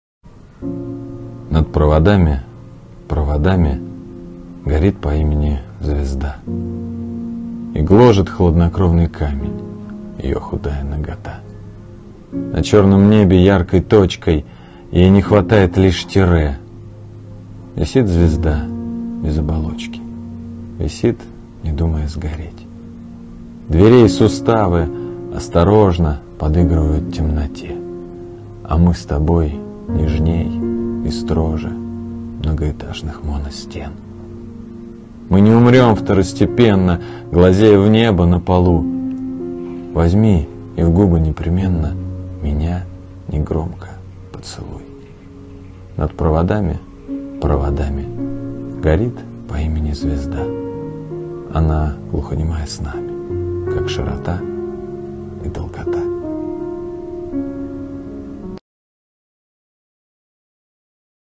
Звезда | Читает автор: Андрей Медведев
Над проводами, проводами
Висит по имени звезда,
И гложет хладнокровный камень
Её худая нагота.

На чёрном небе яркой точкой
(Ей не хватает лишь тире)
Висит звезда без оболочки,
Висит, не думая сгореть.

Дверей суставы осторожно
Подыгрывают темноте,
А мы с тобой нежнее, строже
Многоэтажных моностен.

Мы не умрем второстепенно,
Глазея в небо на полу.
Возьми и в губы непременно
Меня негромко поцелуй.

Над проводами, проводами
Она глухонемая с нами,
Как широта и долгота.